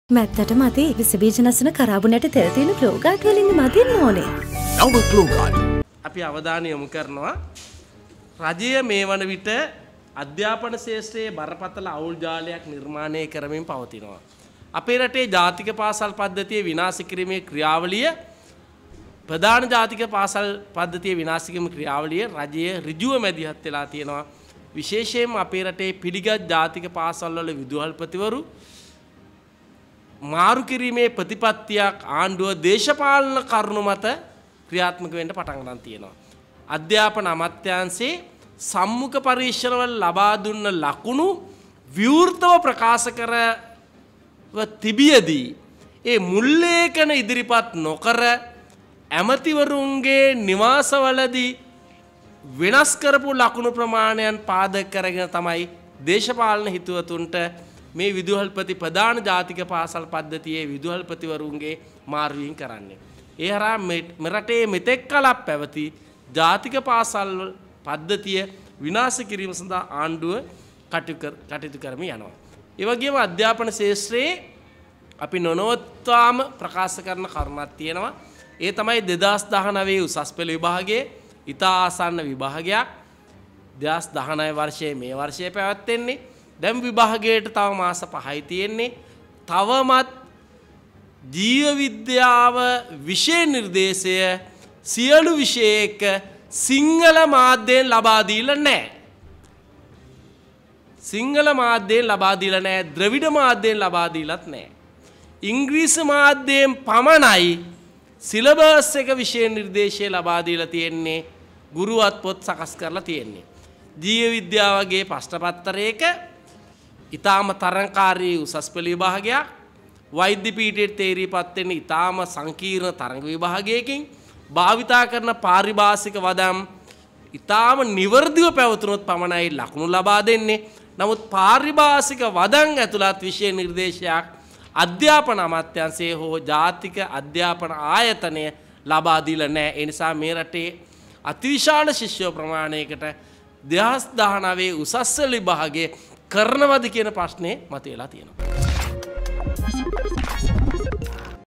ජීව විද්‍යා විෂය නිර්දේශය සියලු විෂය එක්ක සිංහල මාධ්‍යයෙන් ලබාදීලා නැ | Breaking.lk
ජීව විද්‍යා විෂය නිර්දේශය සියලු විෂය එක්ක සිංහල මාධ්‍යයෙන් ලබාදීලා නැ